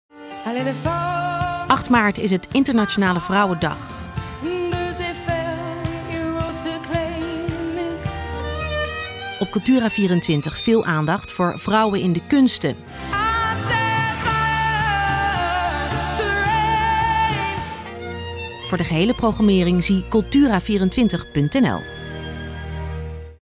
THEMA: Vrouwen in de kunsten
8 maart is het Internationale Vrouwendag. Daarom zetten we "Vrouwen in de Kunsten" centraal. Van Adele tot Versalis, van Janine tot Abramovic; van 4 tm 8 maart iedere avond op Cultura24.